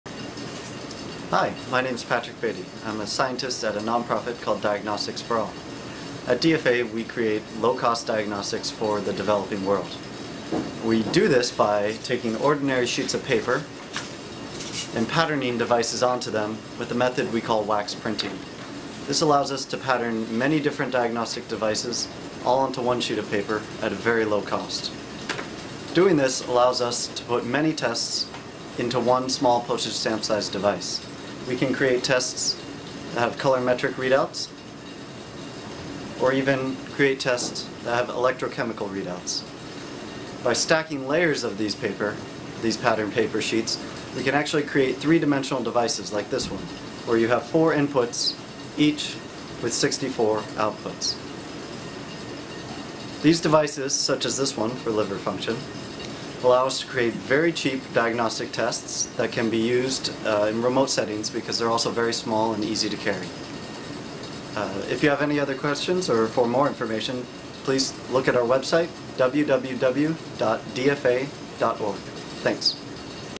Diagnostic For All - a diagnostic lab on printer paper
Diagnostics For All, a Massachusetts non-profit, earned top honors at the WHCC Affordable Health Innovations Exhibit and Awards Program in Washington D.C. in April, 2010 for its work on a diagnotsic tool that uses postage stamp-sized pieces of comercial printing paper.